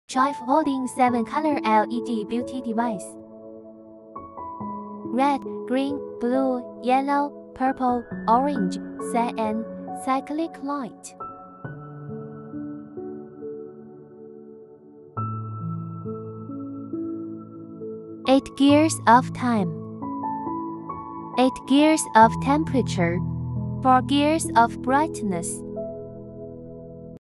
Konmison LB463 Tri-folding 7-color LED Beauty Device
Tri-folding 7-colors LED Beauty Device Functions:
Reduce wrinkles
Brighten skin
Lighten spots
Remove acne
Shrink pores
Increase skin elasticity
Reduce oil secretion